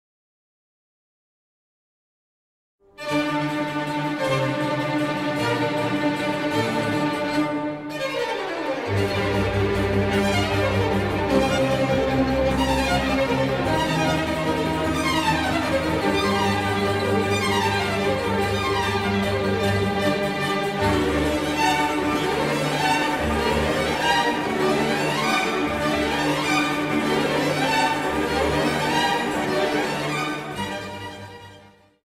Strawberry Moon timelapse on Penna furnace 05/06/2020
Strawberry Moon timelapse on Penna furnace, an ancient brick factory in Sampieri, a little town in Sicily.